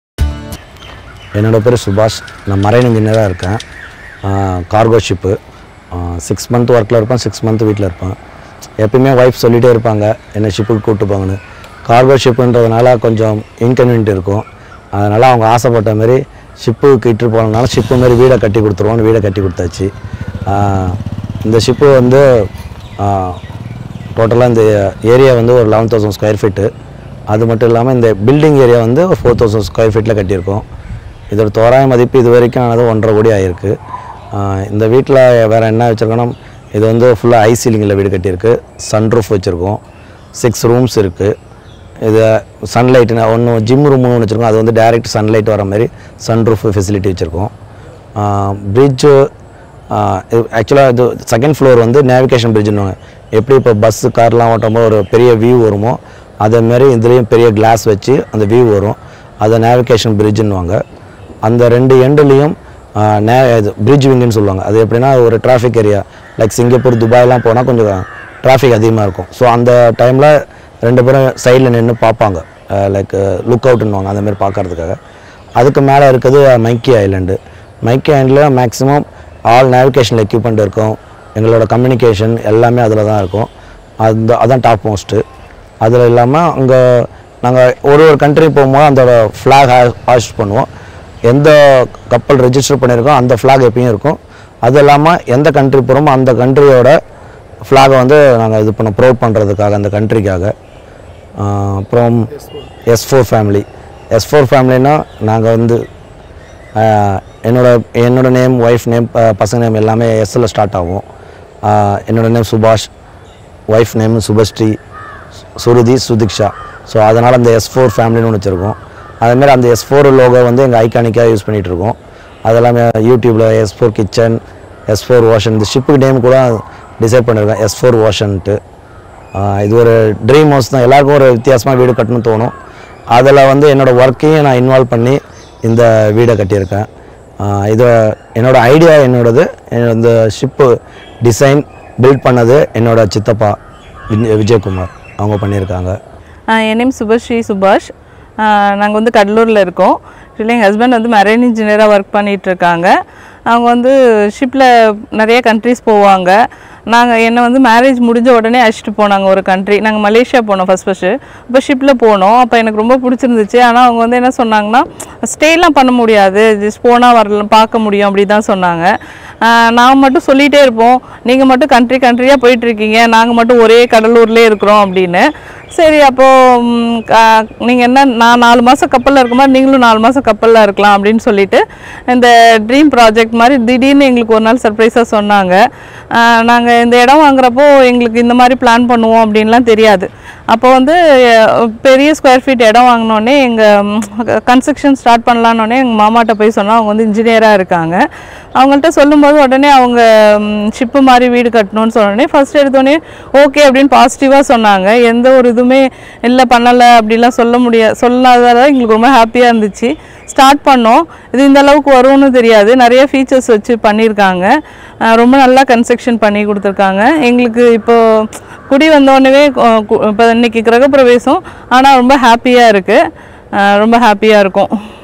Ship Shaped House | மனைவி ஆசைக்காக கப்பல் வீடு கட்டி அசத்திய மரைன் இன்ஜினியர் | Marine Engineer
மனைவி ஆசைக்காக கப்பல் வீடு கட்டி அசத்திய மரைன் இன்ஜினியர்

News18TamilNadu | Cuddalore | Tamil News